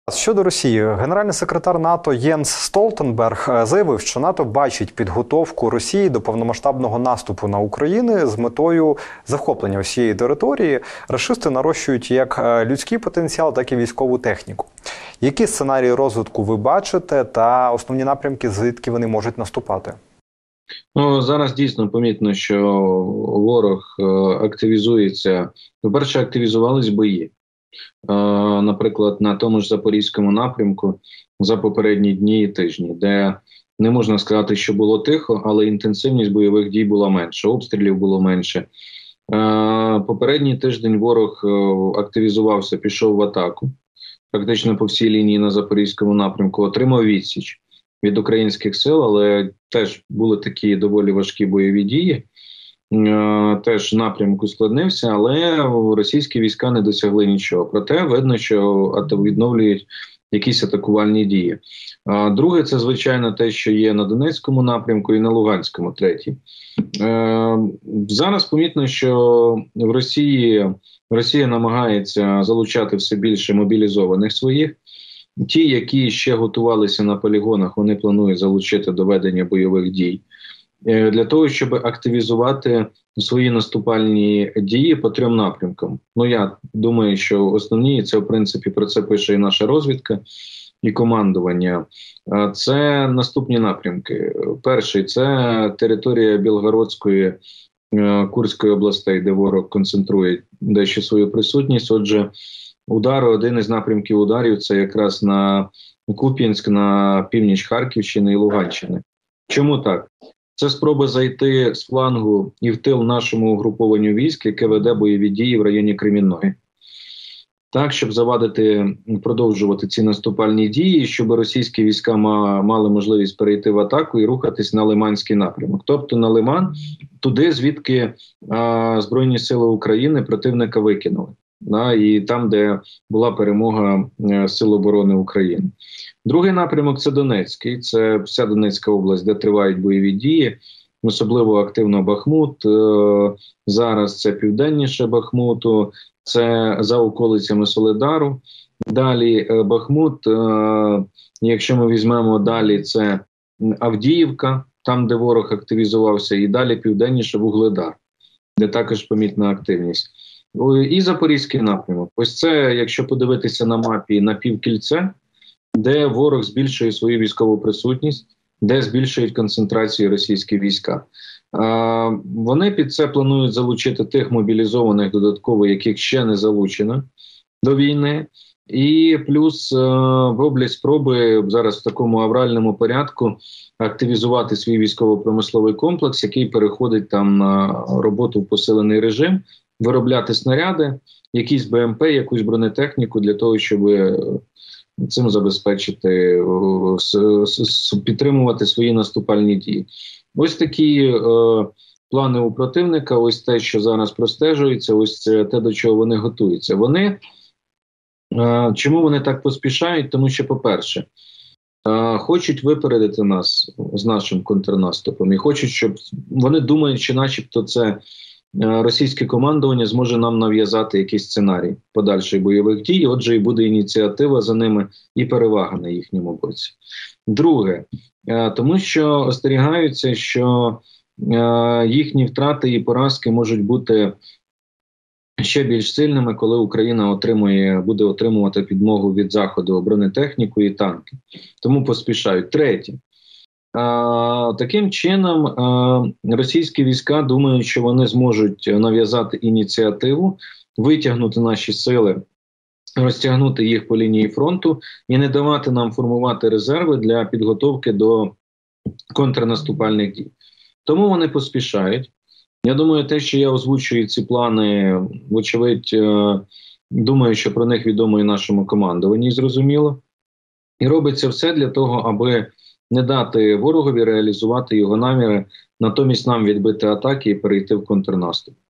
❗️Росія НАСТУПАТИМЕ одразу на 3 напрямках! | Мусієнко
Директор центру військово-правових досліджень Олександр Мусієнко розповів про можливий російський наступ